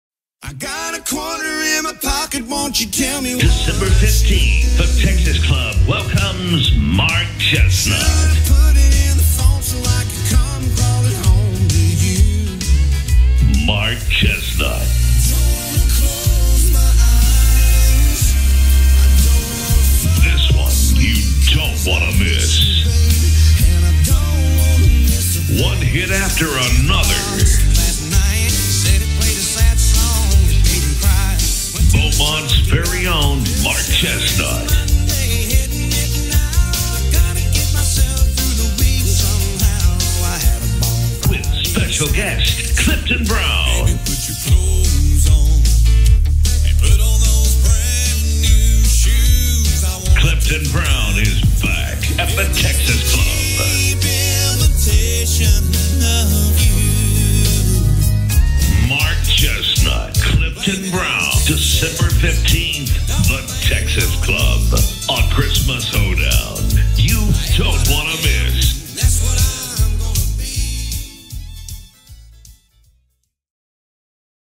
Mark Chesnutt at The Texas Club
Mark Chesnutt and Clifton Brown are back at The Texas Club on December 15th (2017).